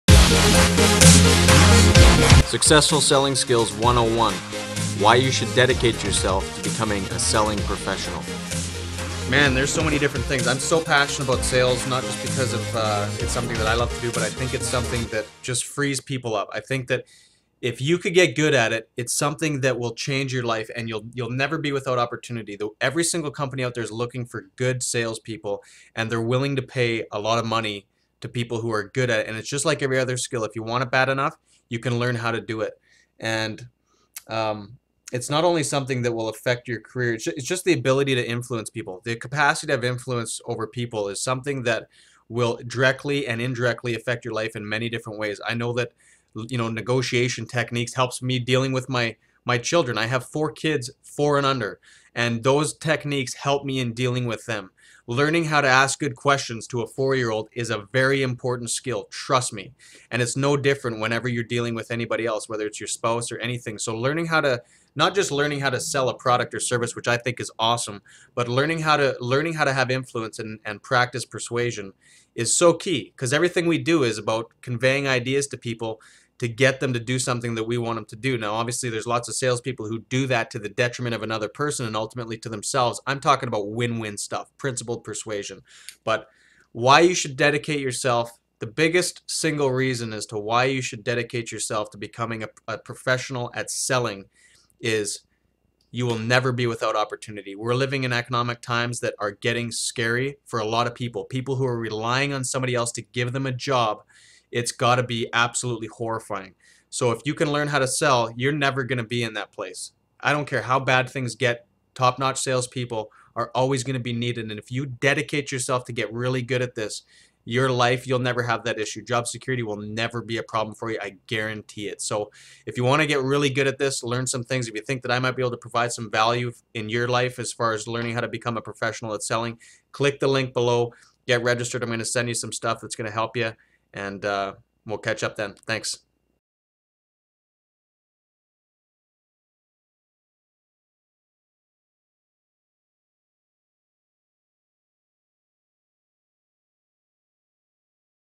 Successful Selling Skills 101 - Why You Should Dedicate Yourself to Becoming a Selling Professional
Good sales people are always and will always be in high demand.  Professional selling is the highest paying profession there is.  Good communication and the ability to influence other people is one the most important skills to develop.  Watch the video - Successful Selling Skills 101 - Why You Should Dedicate Yourself to Becoming a Selling Professional